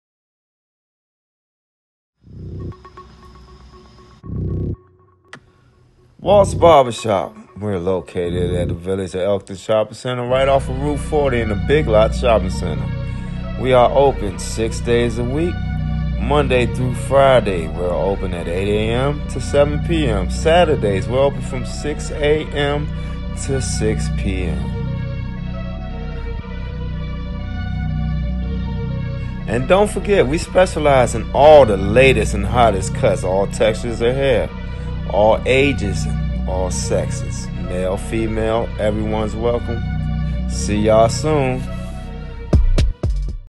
Walts Barbershop in Elkton Maryland
Walt’s Barbershops located at 801 East Pulaski Hwy Elkton Maryland we are open 6 days a week Monday to Saturday unisex including men’s weave, braids, locs and female extensions.

Service women and men Amy ages| all
Is welcome.

Barbercuts 10 point services no long time wait.